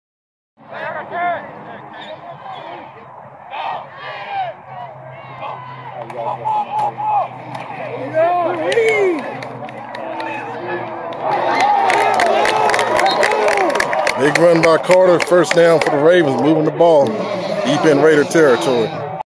Tyshawn Carter Run On 1st Drive
Richmond at Rocky River - Week 1 2016 Season